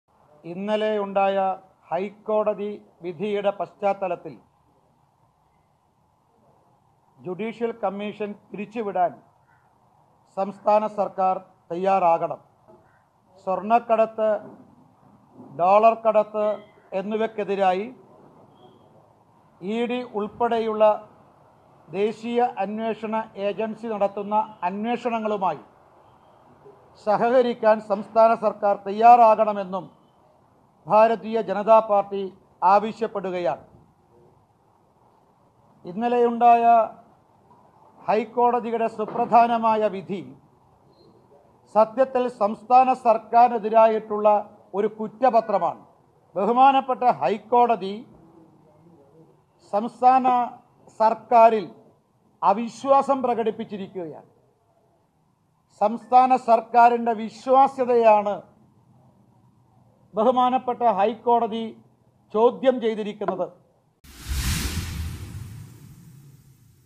"ഡോളർ കടത്തില്‍ മുഖ്യന്‍റെ പങ്ക് വ്യക്തം" |P.K.Krishnadas against CM |ETV Bharat Kerala
സ്വർണക്കടത്ത് കേസിലെ ഹൈക്കോടതി വിധിയുടെ പശ്ചാത്തലത്തിൽ ജുഡീഷ്യൽ കമ്മീഷൻ പിരിച്ചുവിടാൻ സംസ്ഥാന സർക്കാർ തയാറാകണമെന്ന് ബിജെപി ദേശീയ നിർവാഹക സമിതി അംഗം പി.കെ കൃഷ്‌ണദാസ്.